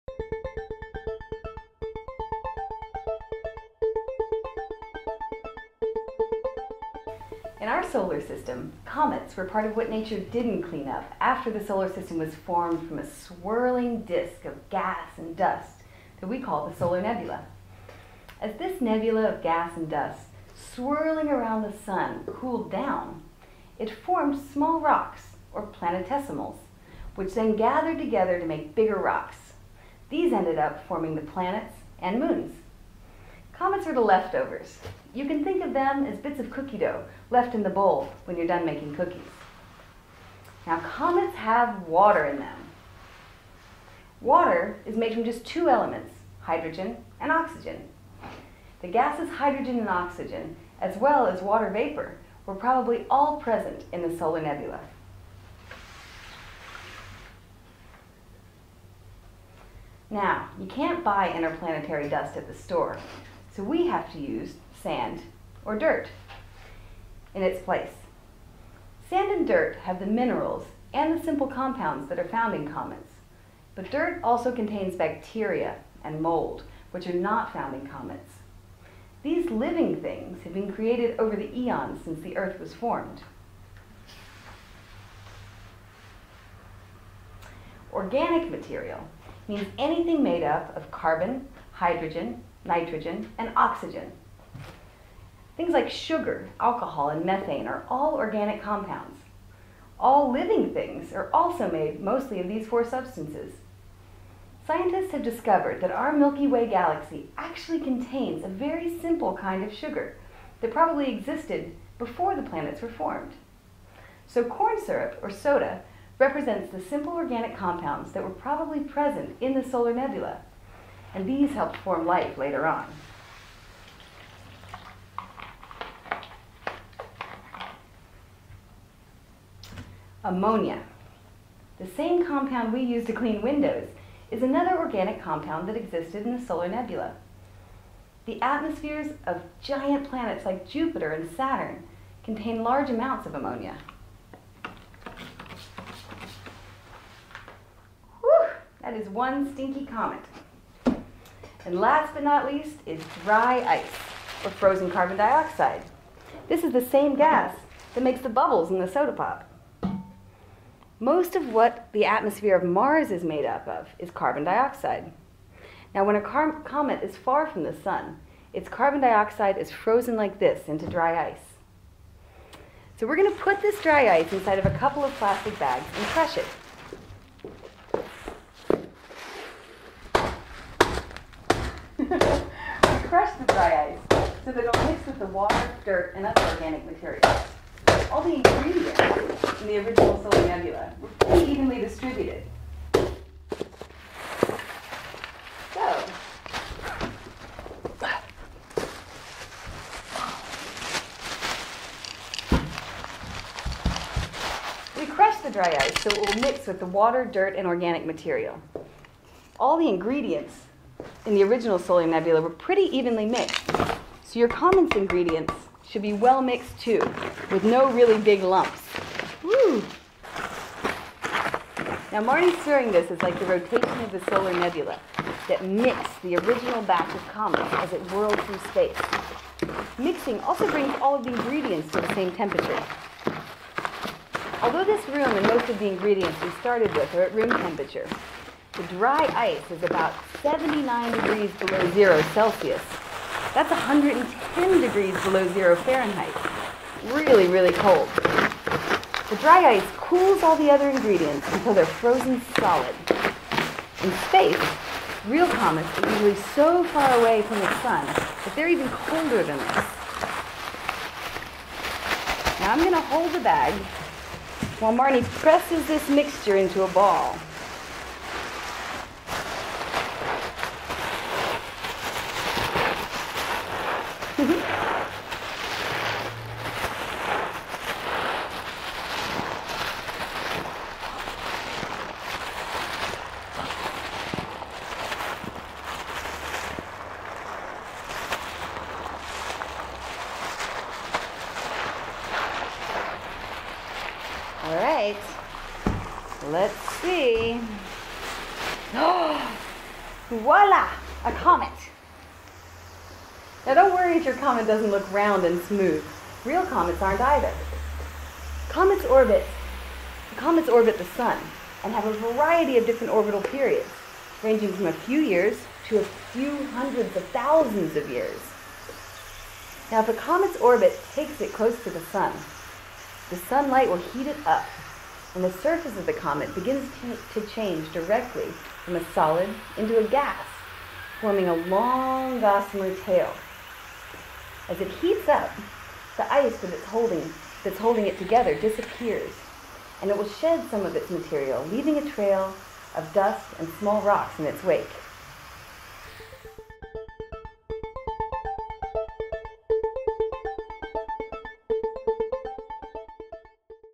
Cook Up a Comet
The Cook Up a Comet activity gives insight into the "dirty snowball" model of comets- composed of material from the early solar system in the form of frozen water and gases, simple organic compounds and dust. Using dry ice and simple household materials, this comet concoction is a big hit with kids and adults alike.